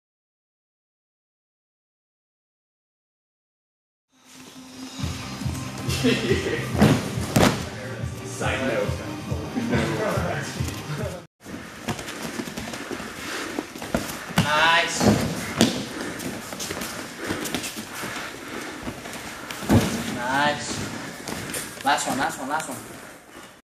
Seaside Jiu-Jitsu Academy - Introductory Takedown Drills
Coach Zach and the guys of the Seaside Jiu-Jitsu Academy intro class in Astoria practice their takedowns.